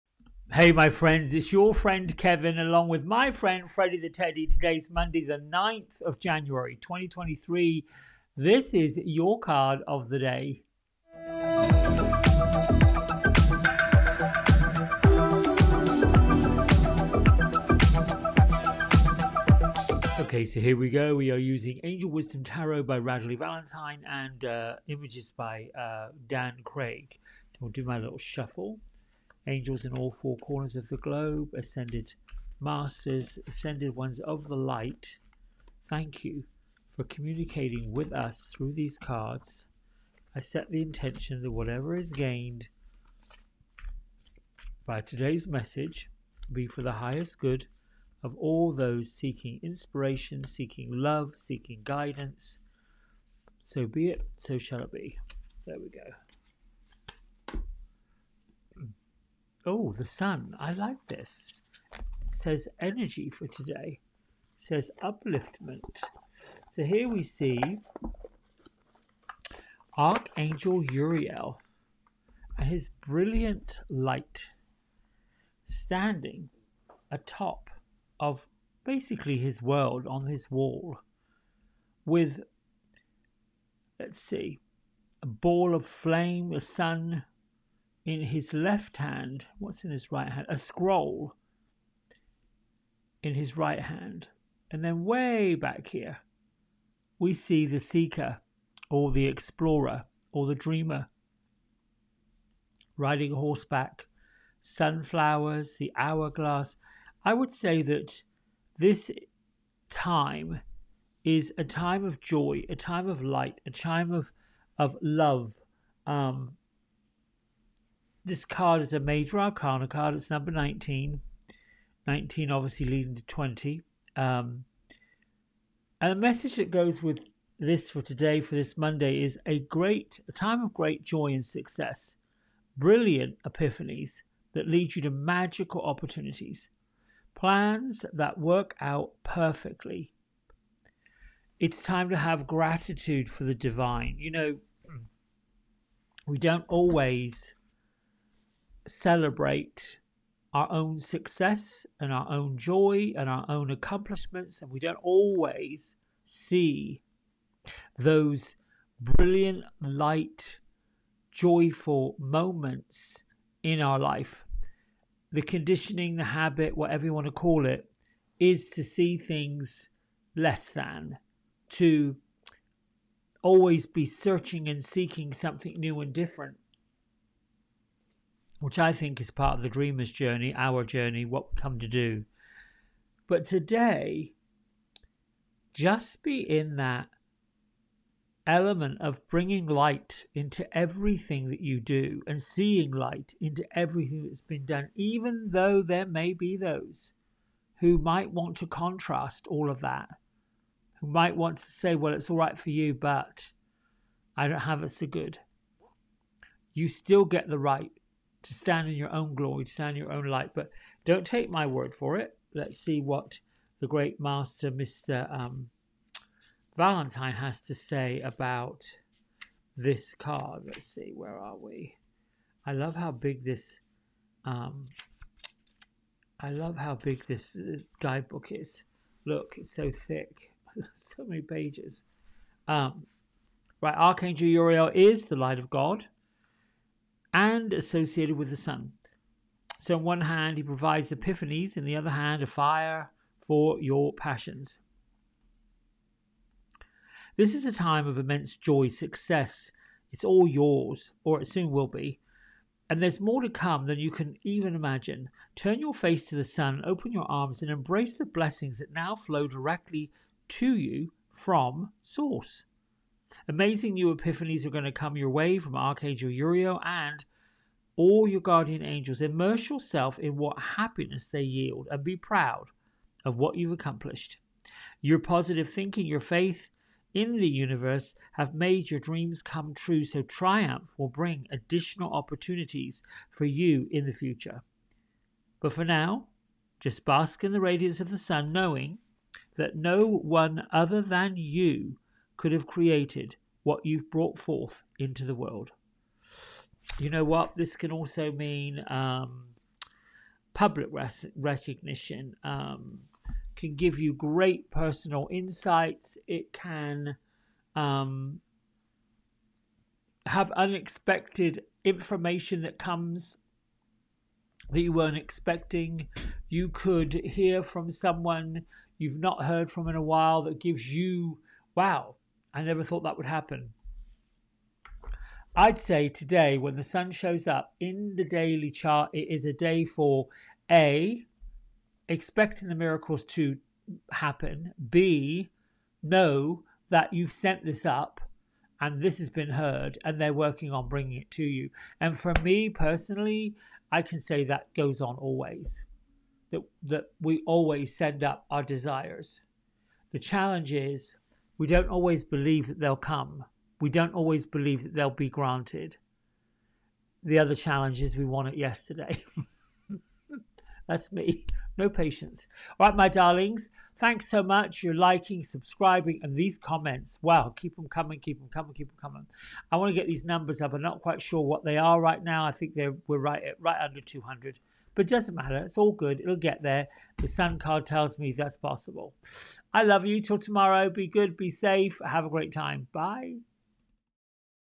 Kev's Card of the Day JANUARY 9 2023
The Sun card brings its energy of great joy and success.  You'll have great epiphanies leading to magical opportunities today.  Don't forget to have gratitude for the Divine.

Card Credit:  Angel Wisdom Tarot by @RadleighValentine